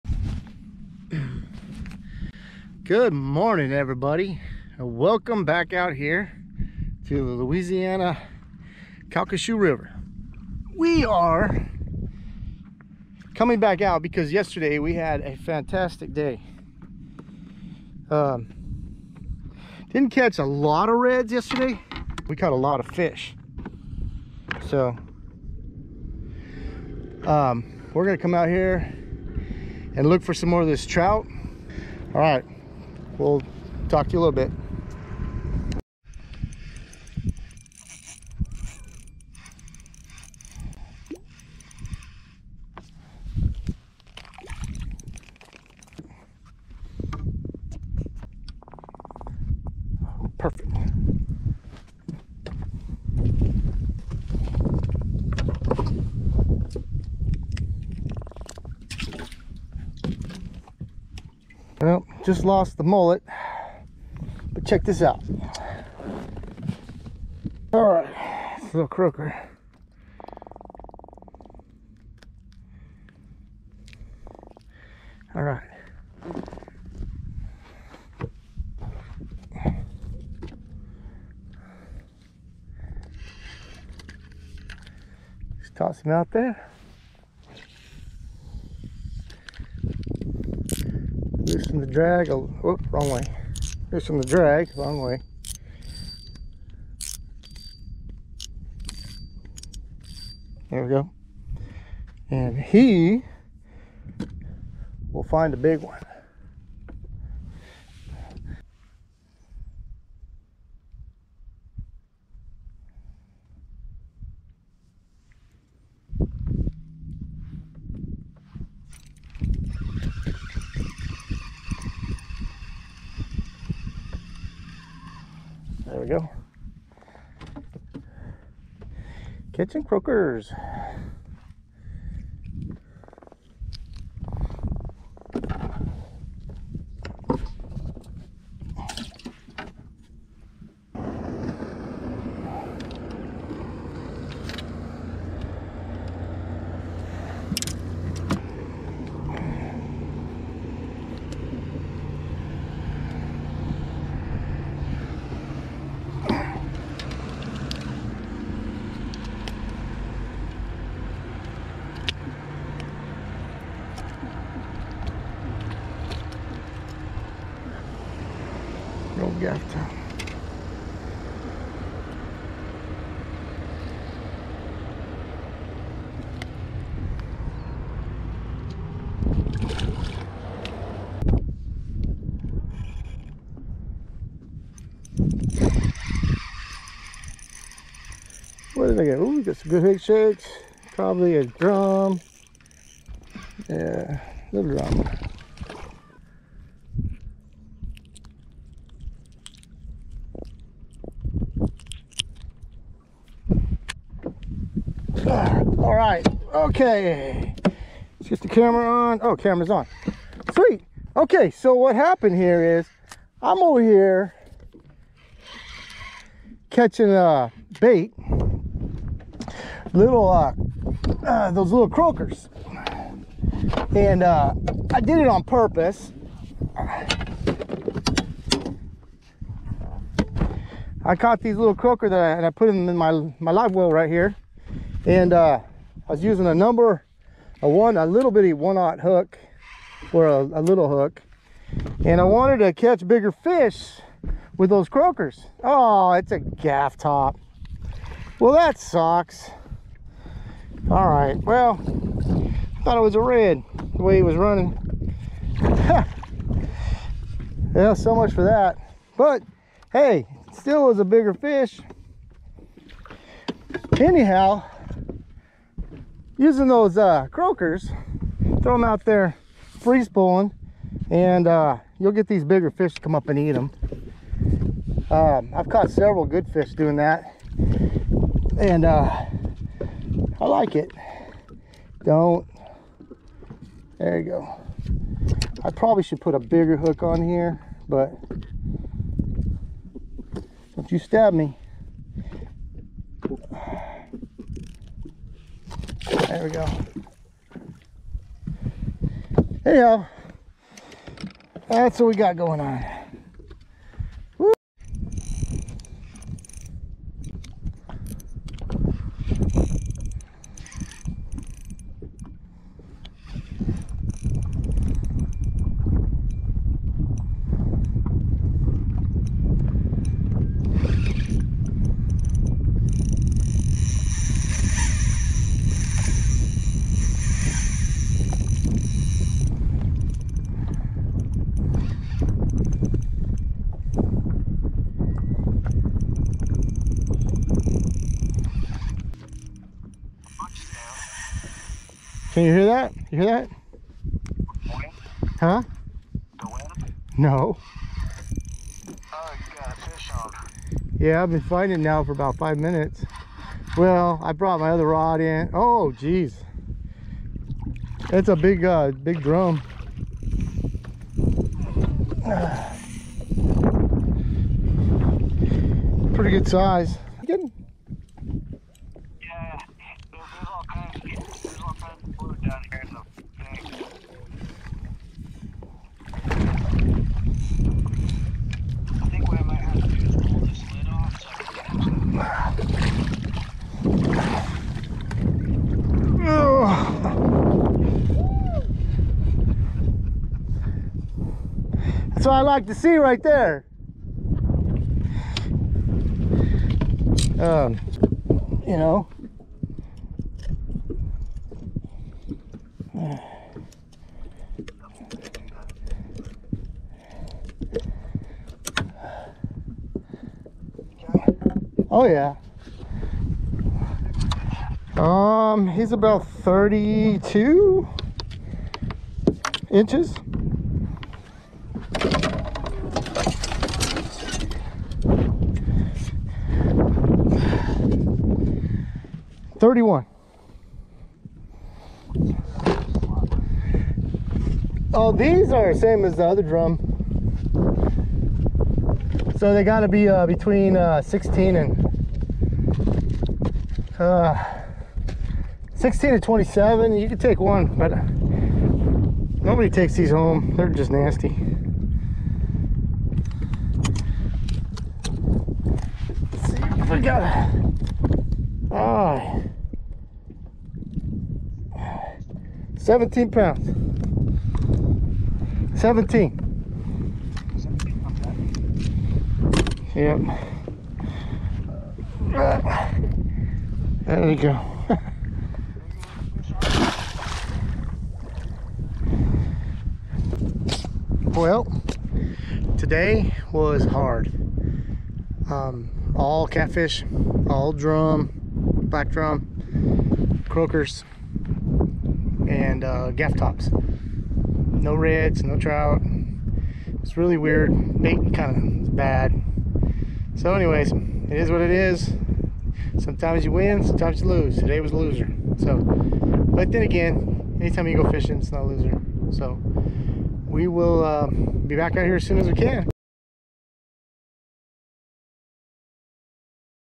Drum and Cats all day
Went out to catch Redfish. Caught some croaker ended up with Black Drum and Gaff Tops.